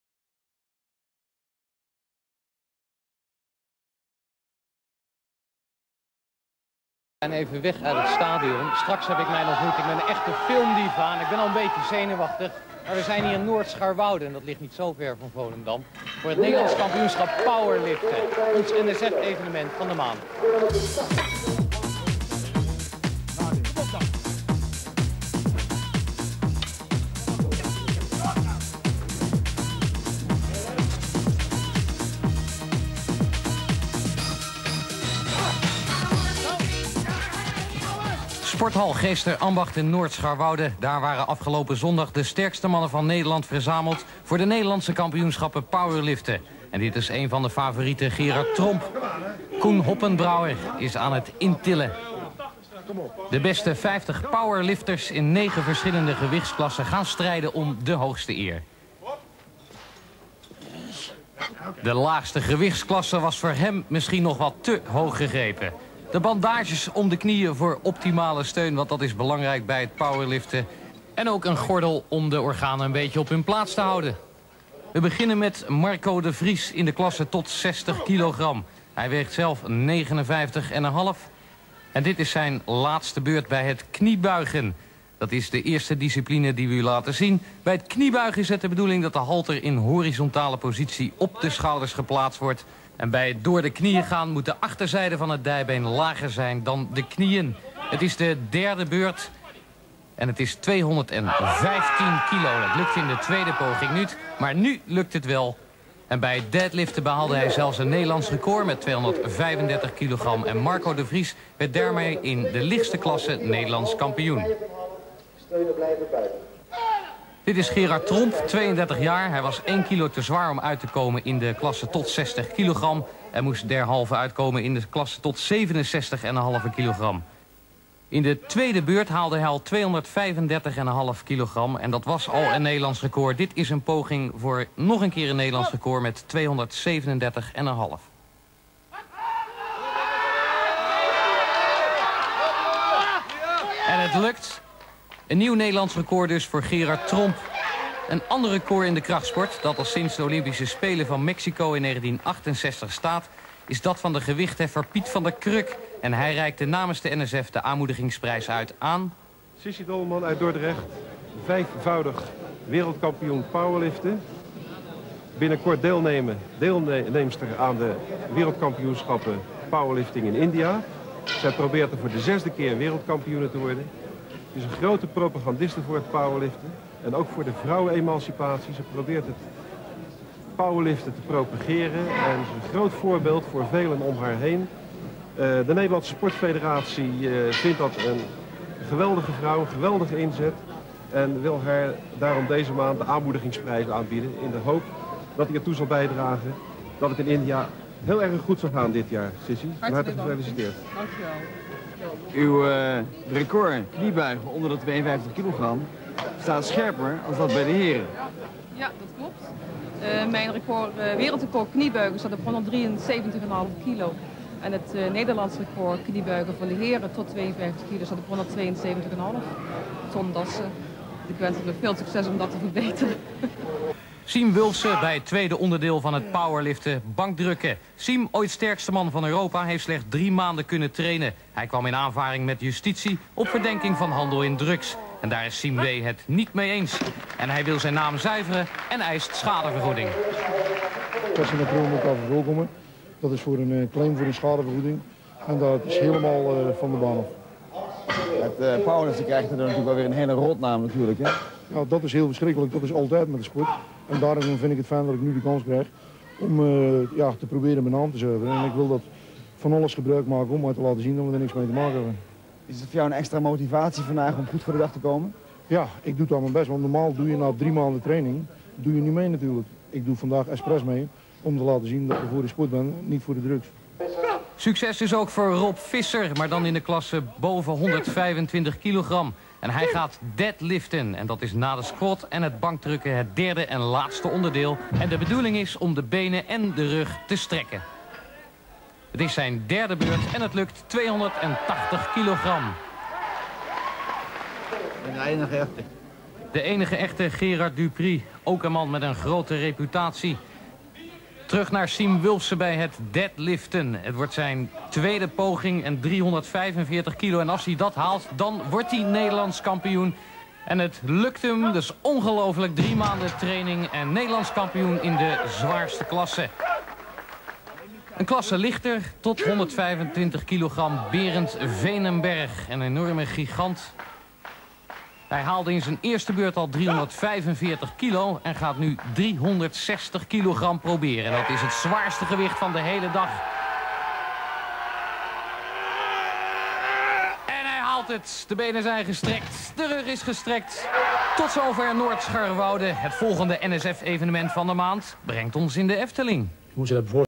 1990 Nederlands kampioenschap powerlifting.
1990 Nederlands kampioenschap powerlifting Veronica sport.
Siem Wulfse.
Piebe Faber.
Gerard Tromp.
Koen Hoppenbrouwer.
Marco de Vries.
Berend Veneberg.
Sisi Dolman.
Rob Visser.
Gerard Duprie.
Piet van der Kruk.

Met dank aan Fred Scholte voor het geleverde beeldmateriaal.